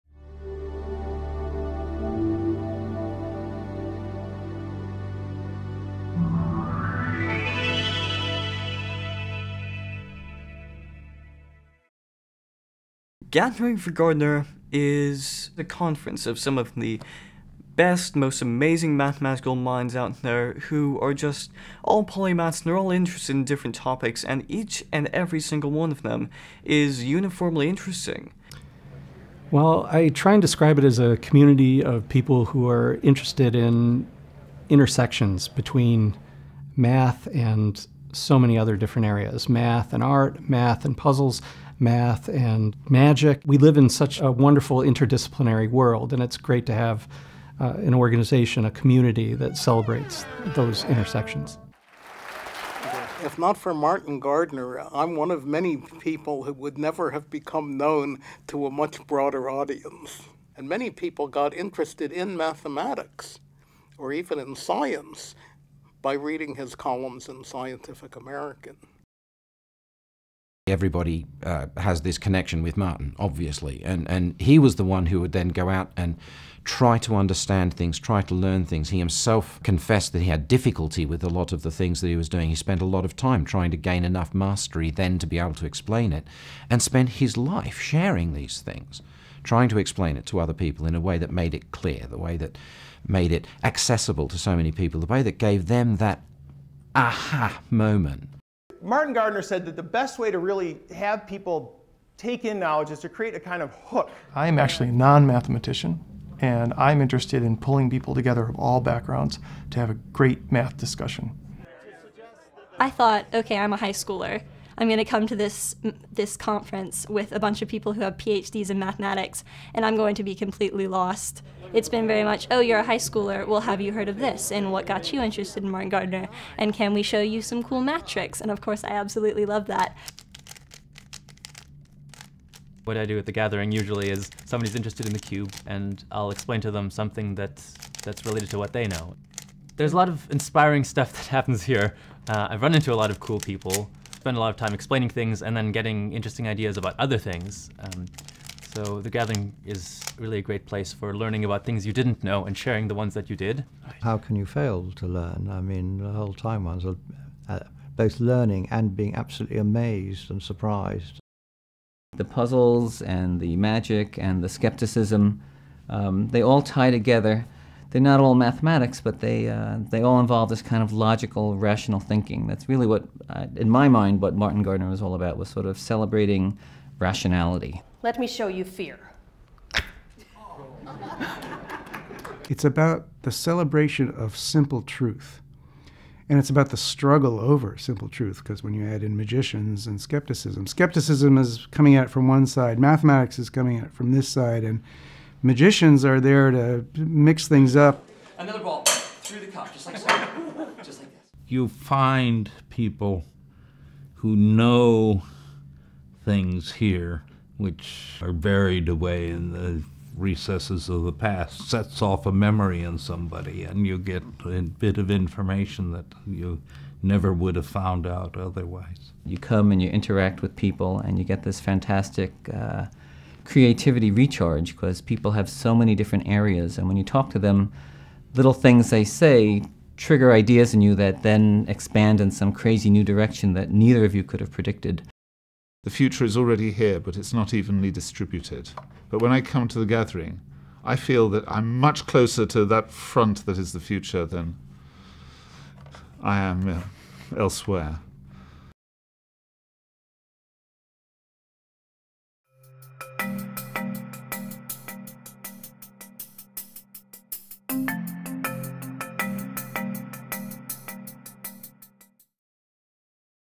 About Gathering 4 Gardner
The Gathering 4 Gardner (G4G) Foundation stimulates curiosity and the playful exchange of ideas and critical thinking in recreational math, magic, science, literature, and puzzles to preserve the legacy of writer and polymath Martin Gardner.